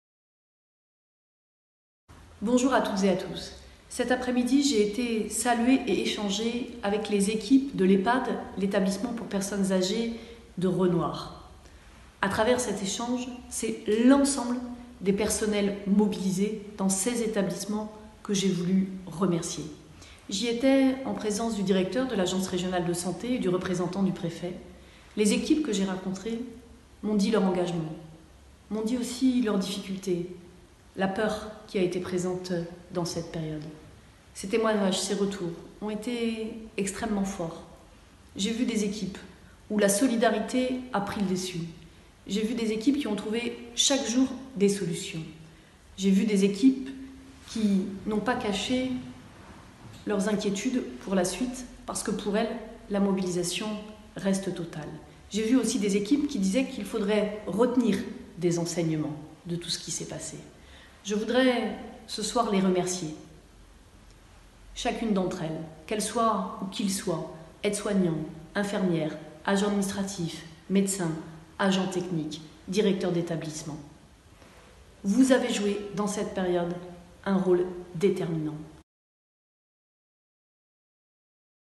Ehpad : à Nantes, vers un déconfinement progressif
Assurer la sécurité des personnes âgées tout en luttant contre l’isolement ou l’ennui, c’est le défi du déconfinement des Ehpad nantais.